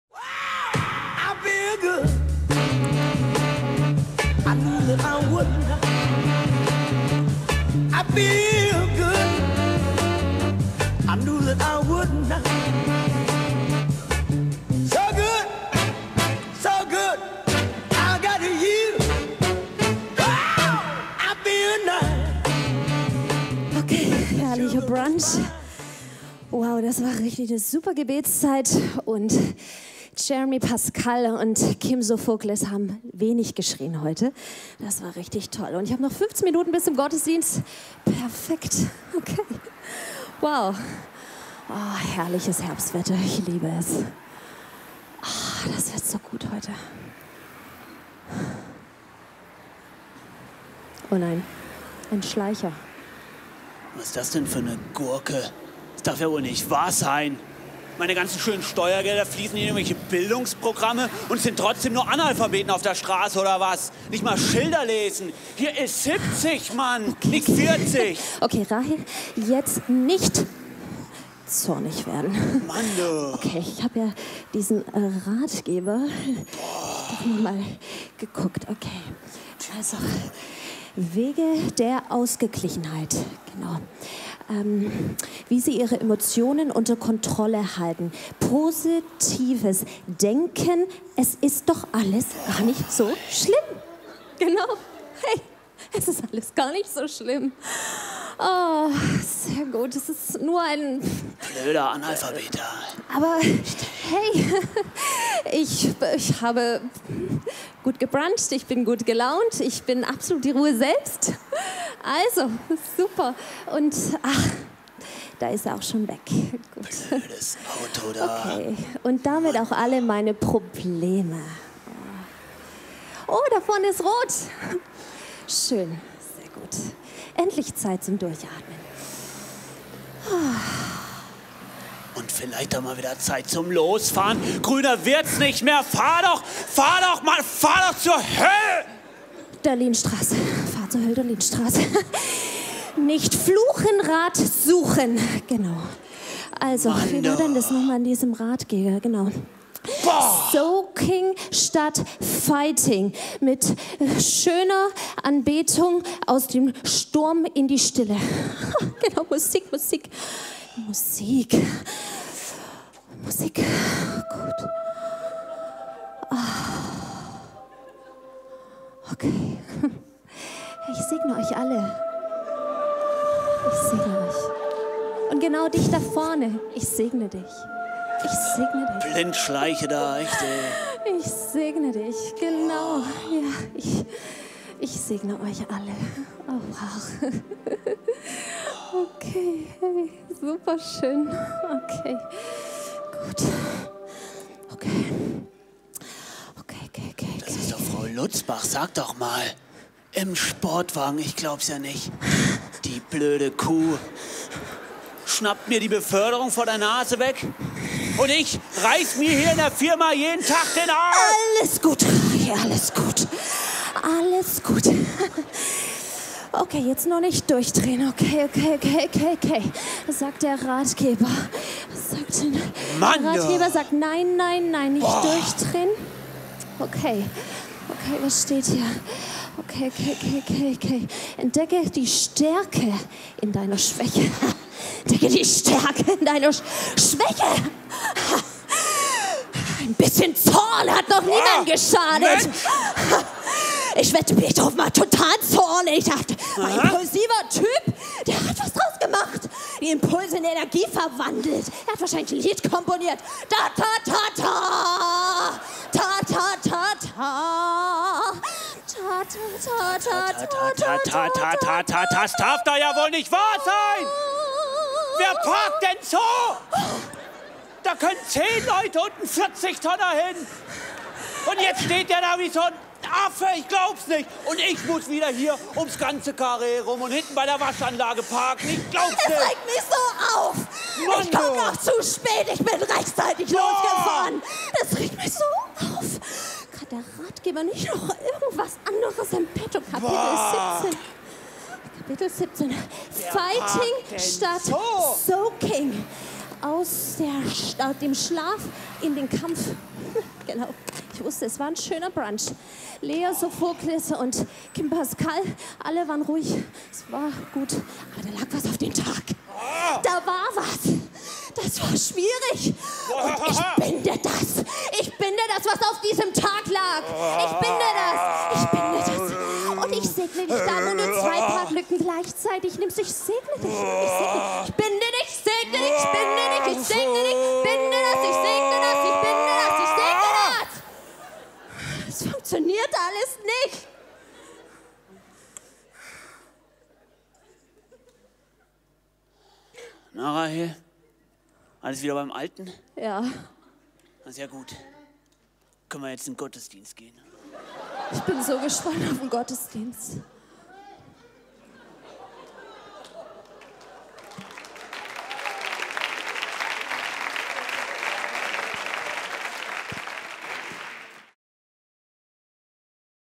TOS Theater Team - "Der alte Mensch und mein frommes Ich"
Ein anschauliches Theaterstück, das die Predigt von Jobst Bittner zum Thema "Neues Leben in Jesus Christus - Das Leben in Freiheit von Sünde (Römer 6)" auf den Punkt bringt.